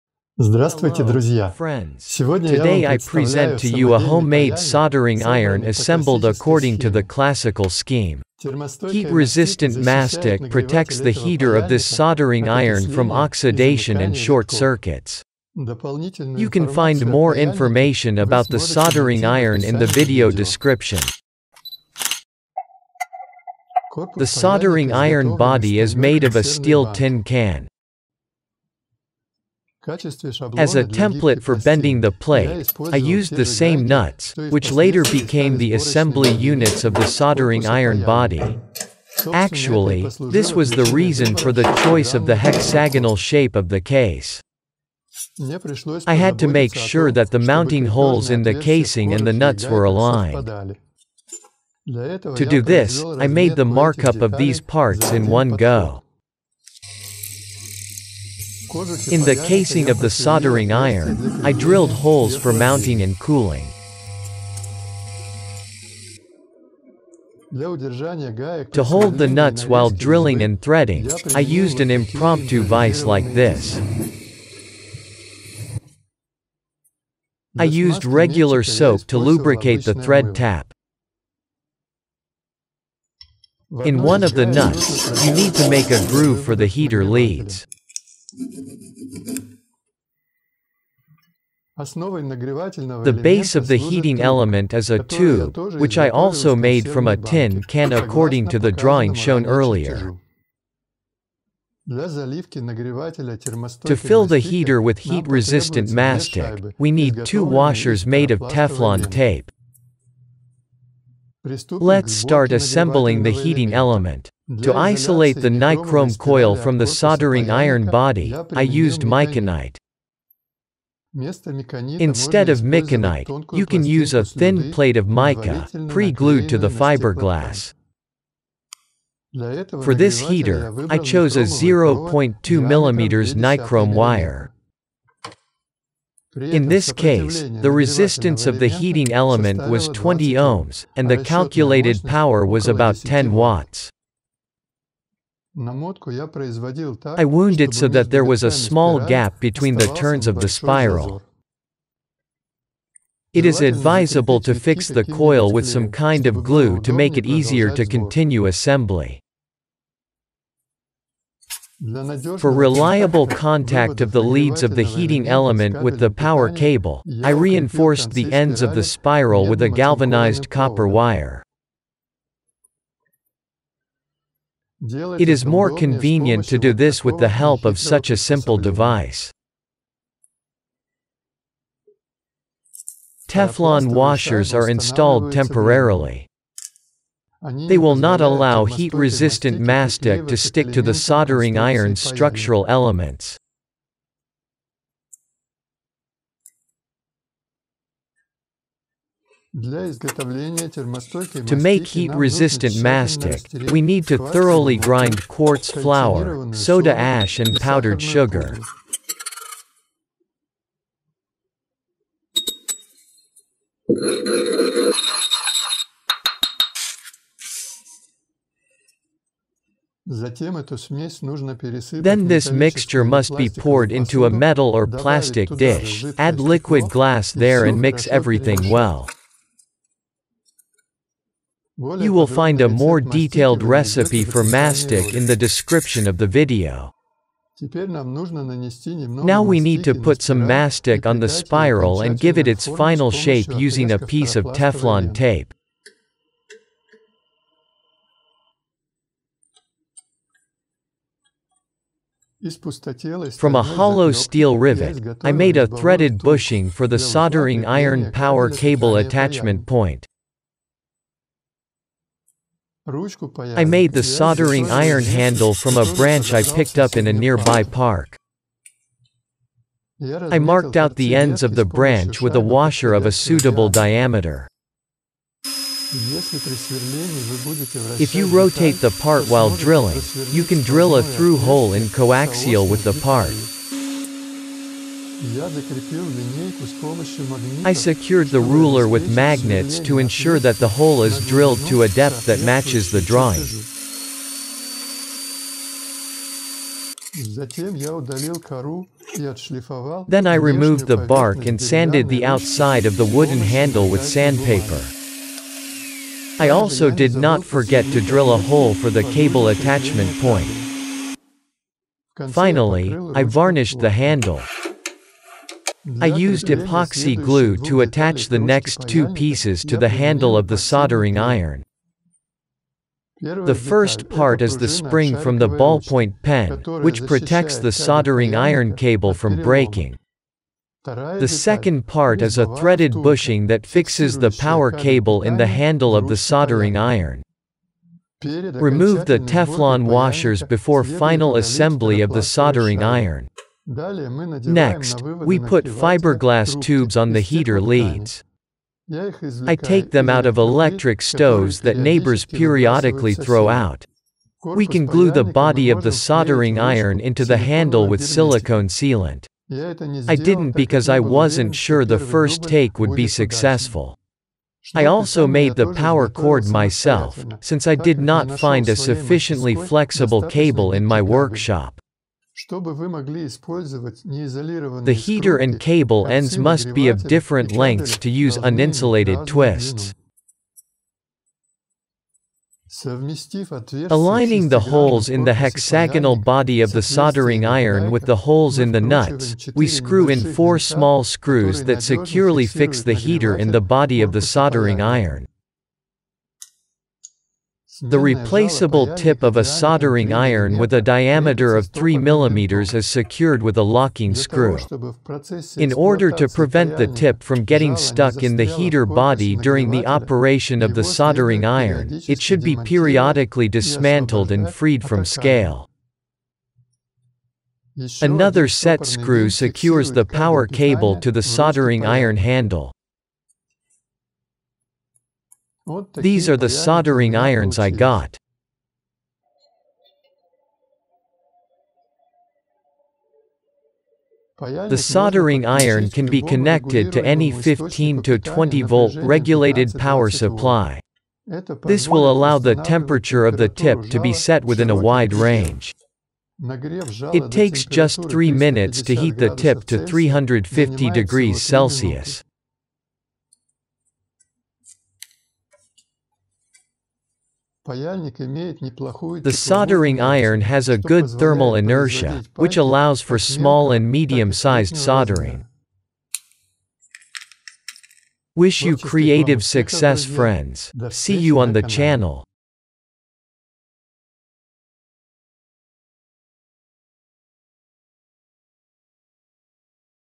DIY Cool Soldering Irons
Source materials:
1. Tin can
2. Nichrome Ø0.2mm
3. Dry branch from the park
4. Locking screw М2х3
5. Locking screw М4х5
6. Screw М1.6х2
7. Nuts М4
8. Fluoroplastic tape
9. Copper wire Ø3mm
10. Spring from a ballpoint pen
11. Silicon dioxide (silicon flour)
12. Liquid glass
13. Caustic soda
14. Powdered sugar
15. Fiberglass tube Ø0.7mm
16. Hollow rivet Ø4mm
17. Epoxy glue
18. Lacquer nitrocellulose
19. Electric cable with plug

Soldering iron technical data:
Nichrome - Ø0.2mm
Resistances 20 Ohm
Voltage - 0 ... 15 Volts
Current - 0.75 Ampere
Power 10 W
350⁰C in 3 minutes

High temperature mastic recipe:
100 parts quartz flour (silicon dioxide SiO2)
4 parts caustic soda (sodium hydroxide NaCO)
2 parts caster sugar
20 parts of liquid glass (silicate glue)

High-temperature mastic drying mode:
1 day at room temperature,
1 hour at a temperature of 100°C,
1 hour at a temperature of 200°C
1 hour at a temperature of 300°C

# soldering # solderingiron